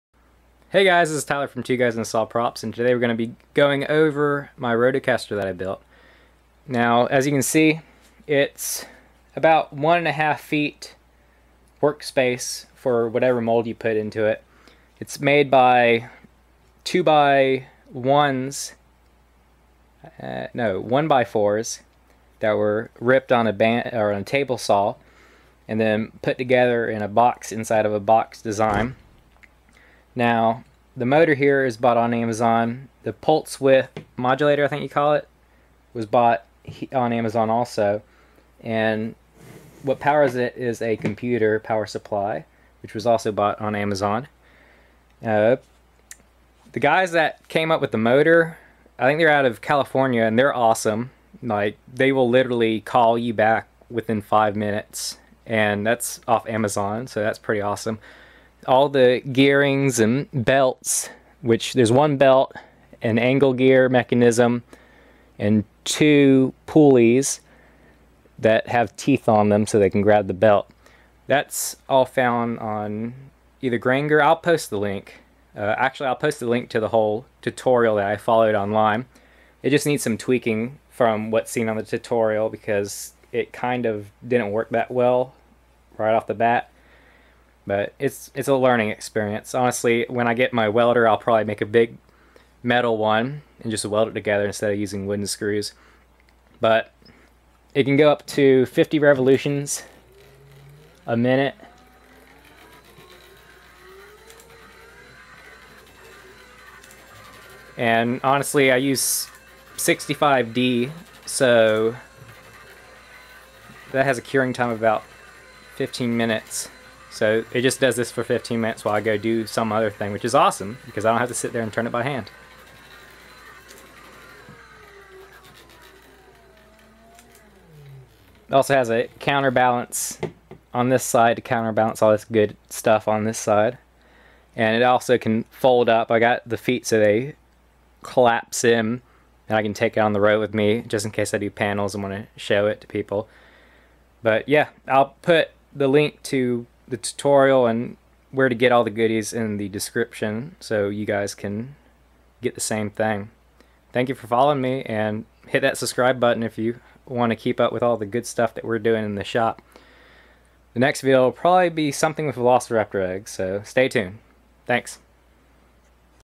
Build Your Own Rotocast Machine
Hey guys we’re on Instagram now. Check out the link below for more videos and a free tutorial in the roto-caster.

Power Supply

Here are the parts ordered from sdp-si.com

A 1M 4-Y24030 30 Teeth, 24DP / Commercial Miter Gear 2

A 6G 3-110037 (XL) Pitch, 110 Teeth, 3/8" Wide, Urethane Belt

A 6Z 3-15DF03710 (XL) Pitch, 15 Teeth, Polycarb Timing Pulley With Aluminum Insert

A 6Z 3-28DF03710 (XL) Pitch, 28 Teeth, Polycarb Timing Pulley With Aluminum Insert